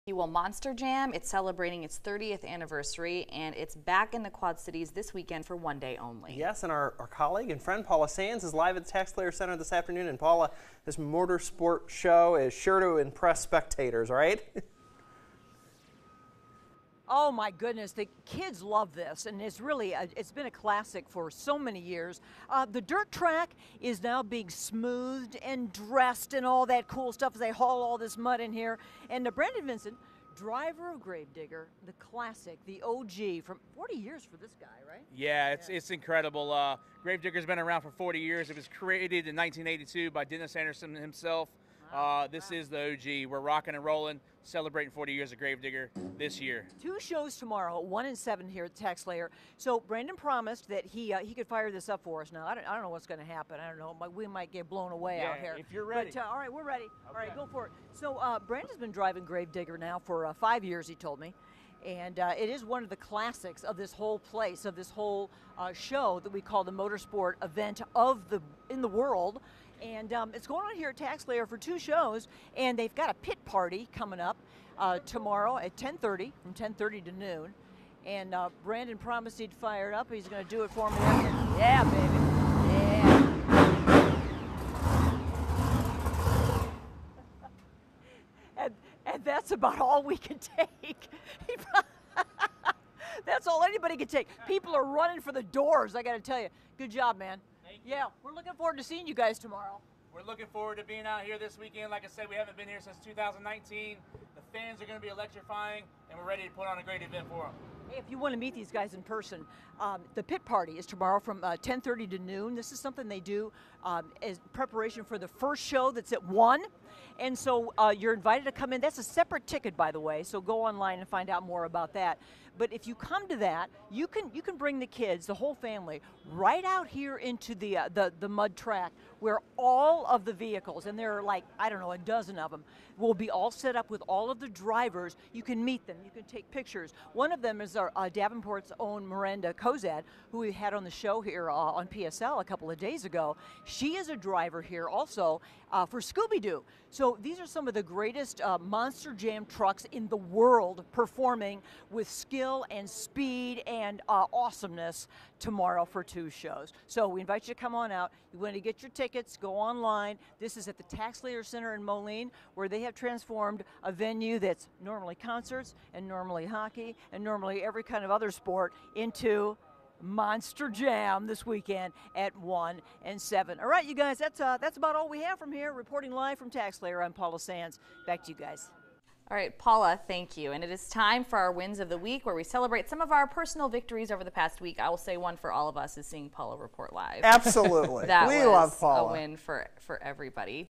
QC Live | Monster Jam Pt 2
April 22, 2022 - One of the world's largest motorsport events, Monster Jam, is coming to the TaxSlayer Center in Moline, Illinois.  KWQC's Paula Sands has a preview of the event.